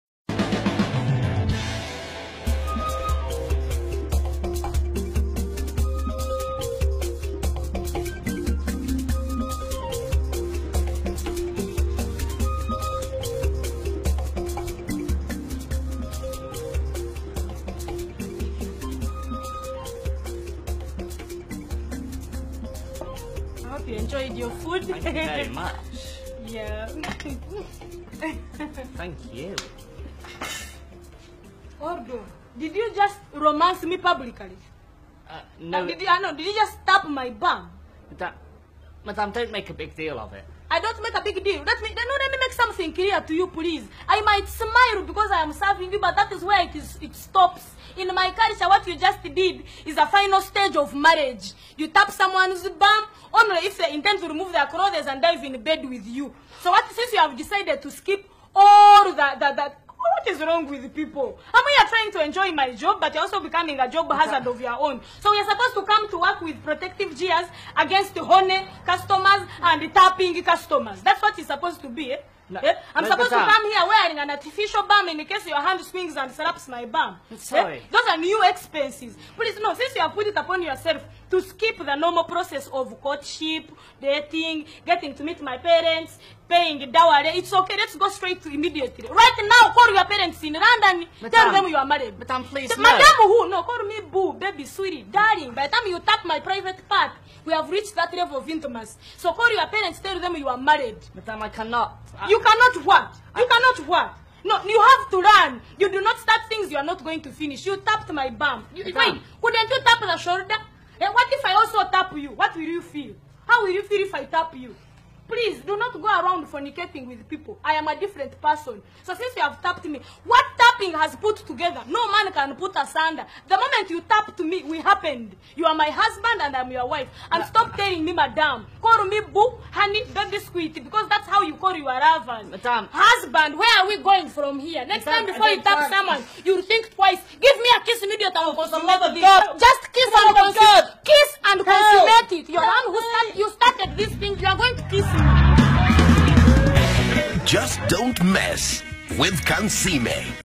Kansiime with Touchy Customer
A Customer With restless fingers Touches Kanslime's In A seemingly joking way. It doesnt go well with kanslime who demands that the man weds her because that is traditionally a marriage proposal...

By Abdallah TripleA

Abdallah Triple A - Productions

            👉 Abdallah Triple A 👈